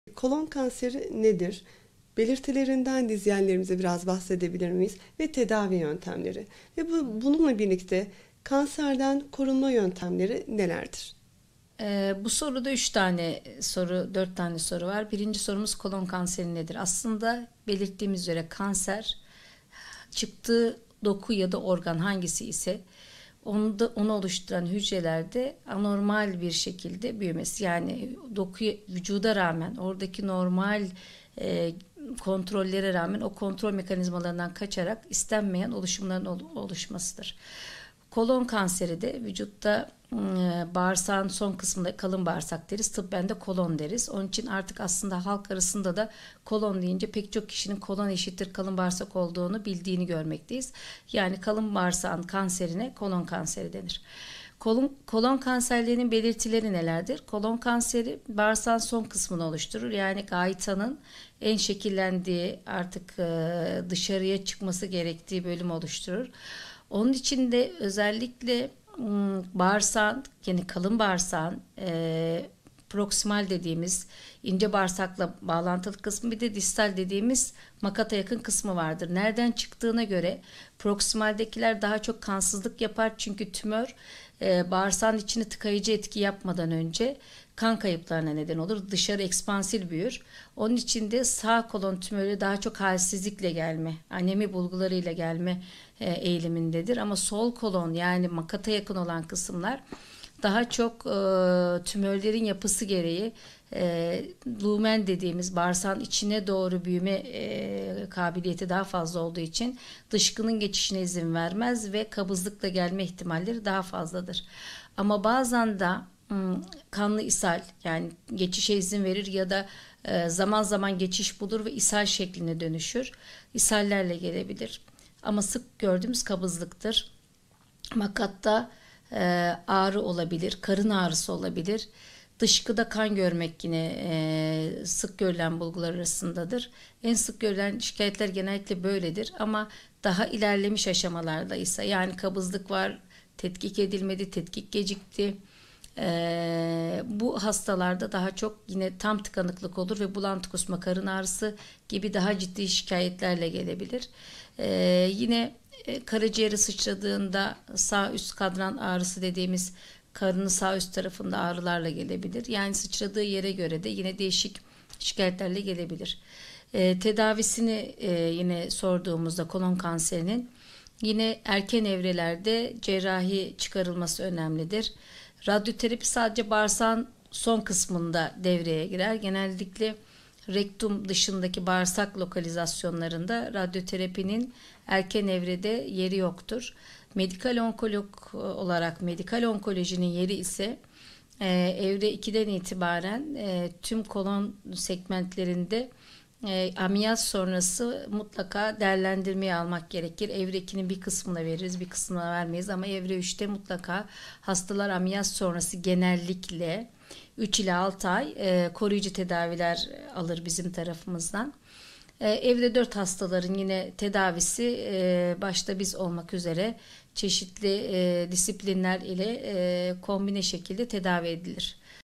İç Hastalıkları Uzm. Ve Medikal Onkoloji Uzm. Doç. Dr. Fatma Şen '' Kolon Kanseri '' Anlatıyor.
Avrasya Hastanesi İç Hastalıkları Uzm. Ve Medikal Onkoloji Uzm. Doç. Dr. Fatma Şen '' Kolon Kanseri '' hakkında detaylı bilgiler veriyor.